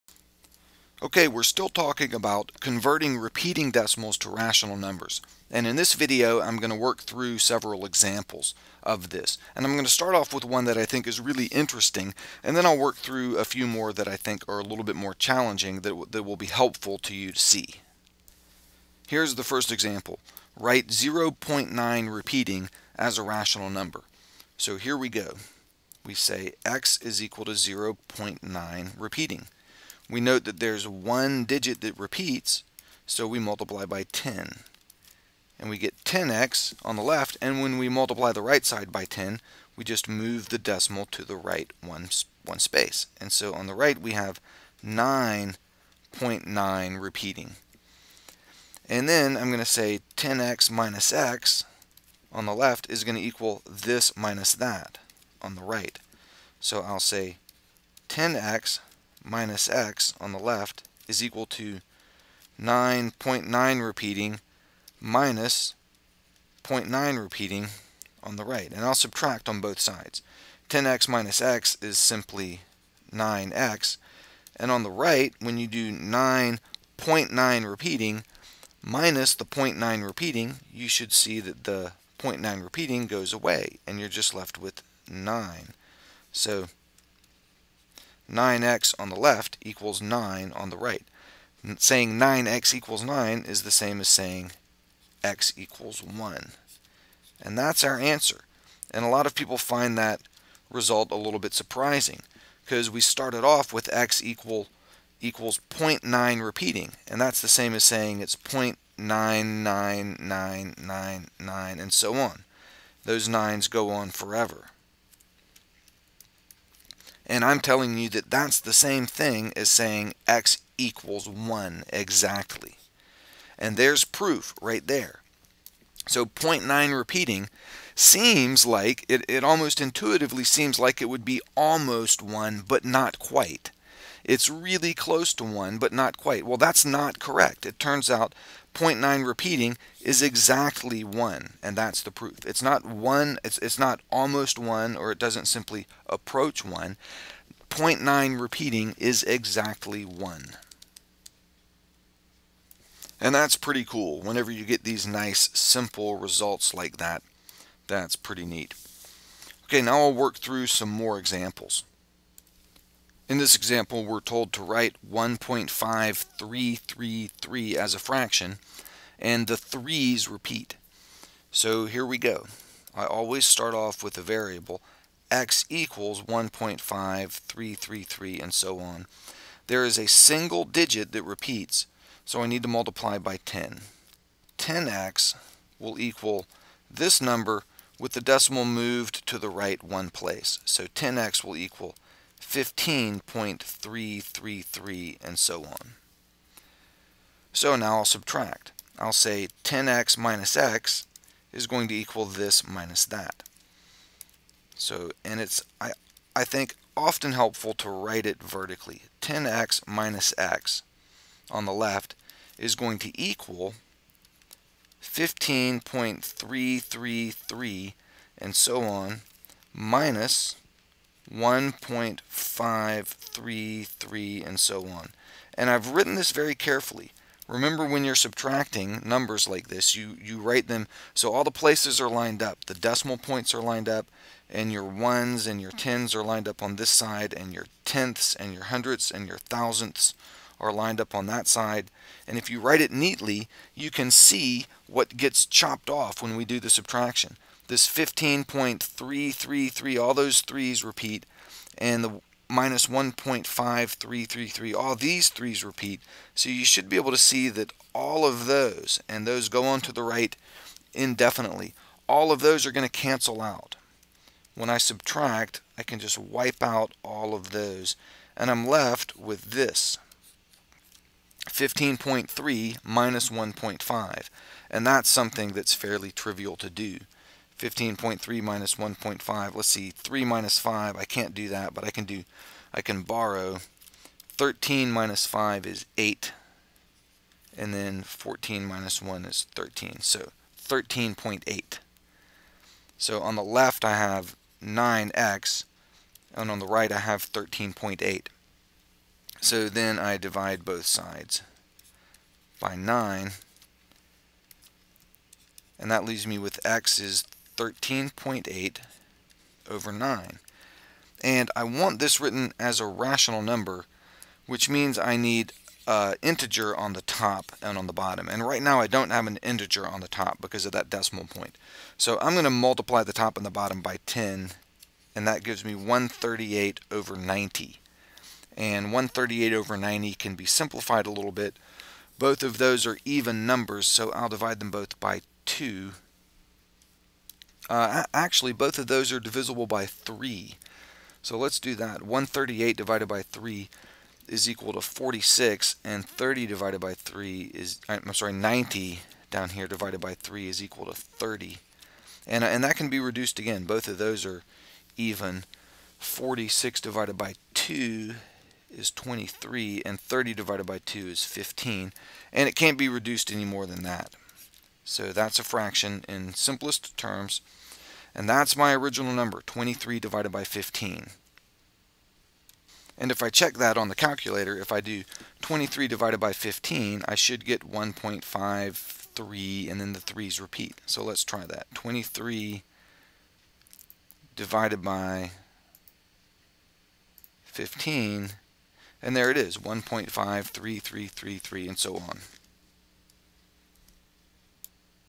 Algebra 1 11.2d - Converting Repeating Decimals to Fractions - Part 2
Converting repeating decimals to fractions.  From the Algebra 1 course by Derek Owens.